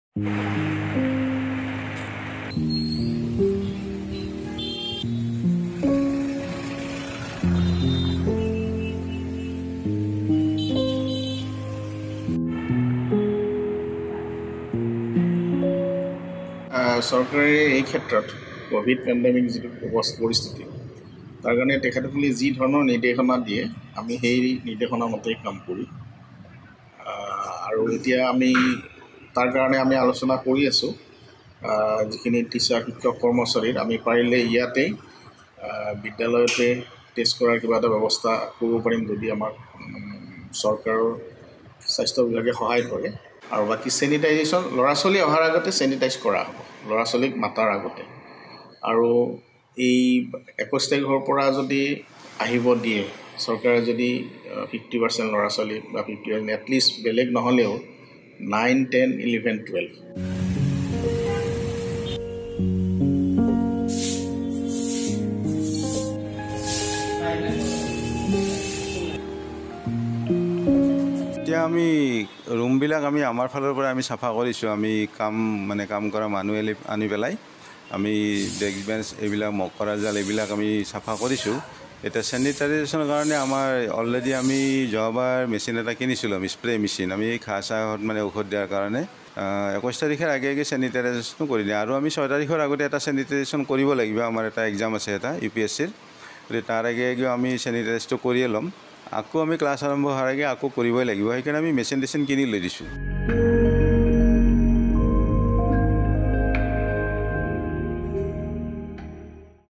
Sanitisation begins in Guwahati Schools
Here is how Guwahati schools are gearing up to re-open from September 21. Most schools have already started cleaning and sanitising the premises. Take a look.